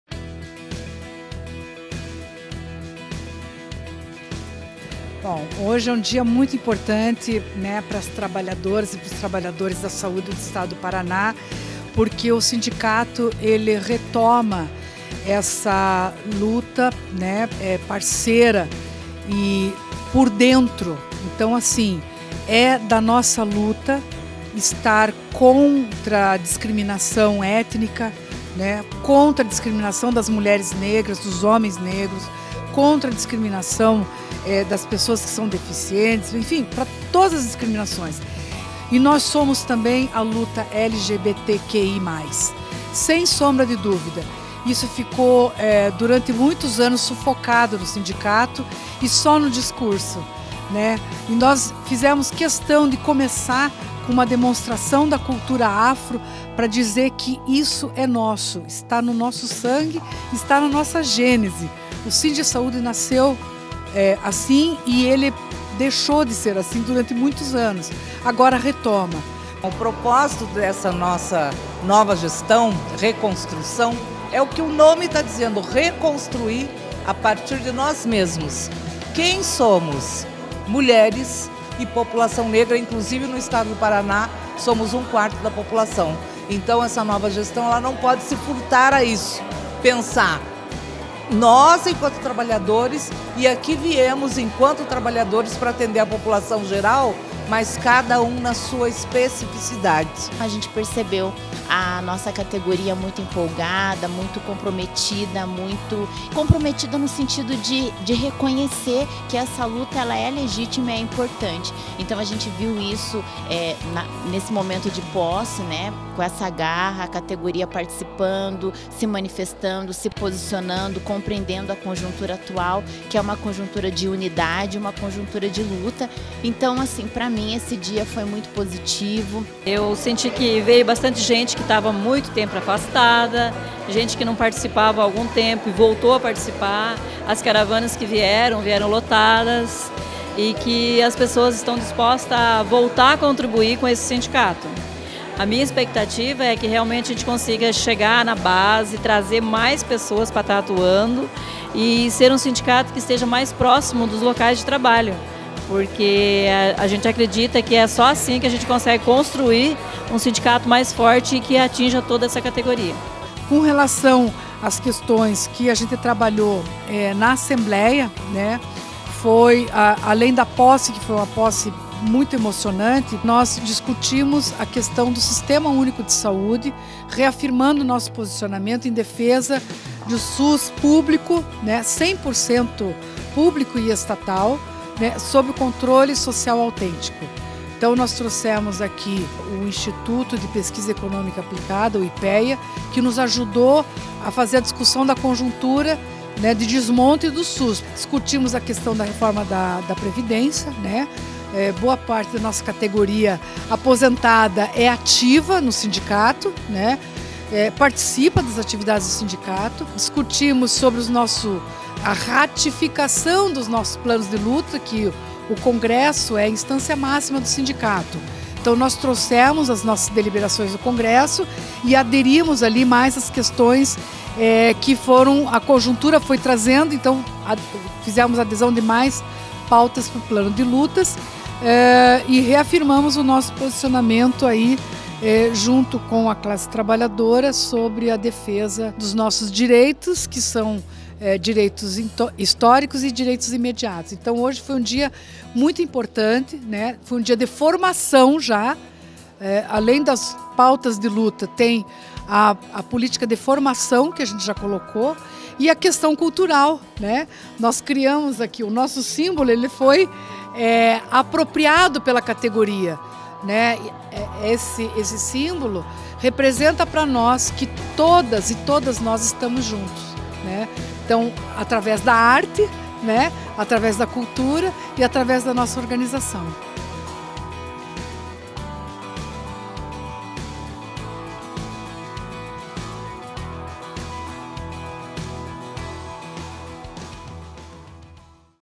Nova diretoria do SindSaúde-PR fará uma gestão inclusiva e combativa
Com discurso firme, focado na participação e união dos trabalhadores, a nova direção do SindSaúde-PR tomou posse neste sábado (1), com o apoio de movimentos sociais e organizações sindicais do estado. Veja como foi: